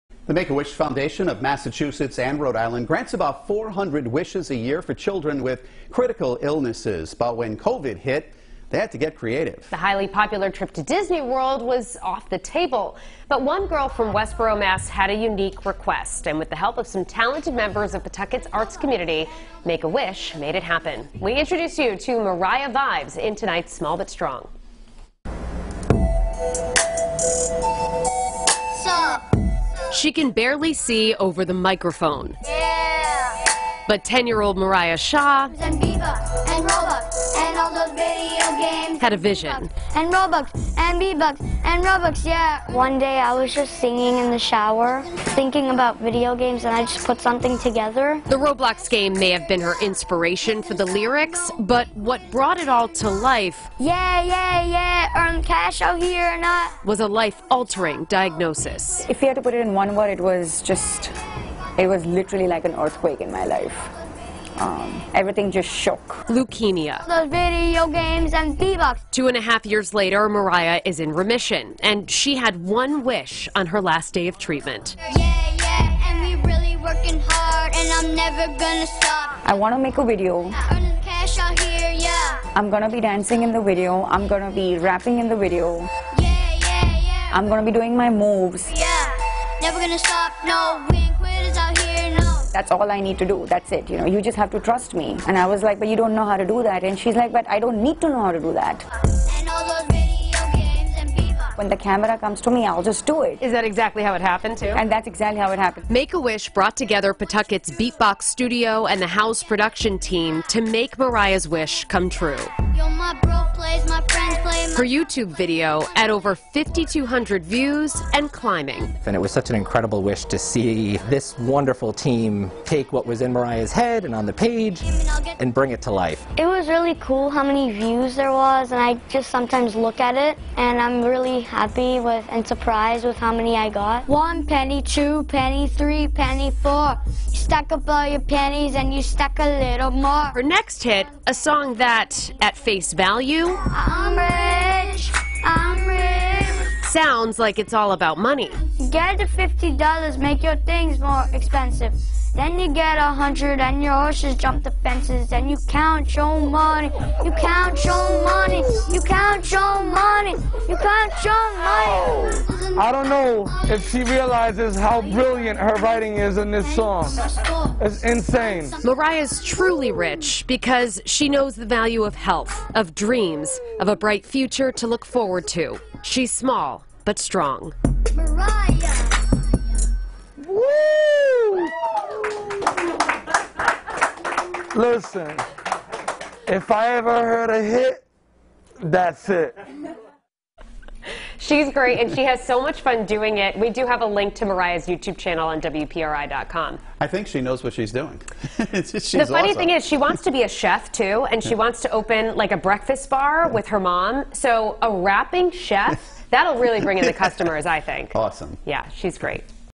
Leukemia survivor’s wish to make rap video comes true thanks to local organizations, artists
Two and a half years ago, Miraya was diagnosed with Acute Lymphocytic Leukemia, according to her mom, Ronak Shah.